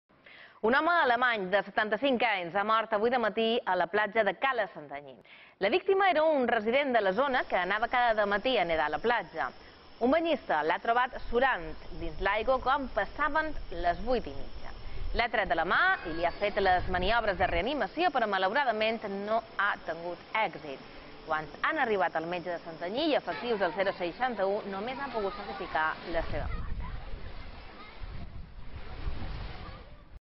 Un home mor a la platja de Cala Santanyí
La víctima era un resident de la zona que anava cada dematí a nedar a la platja. Un banyista l'ha trobat surant dins l'aigua quan passaven les vuit i mitja.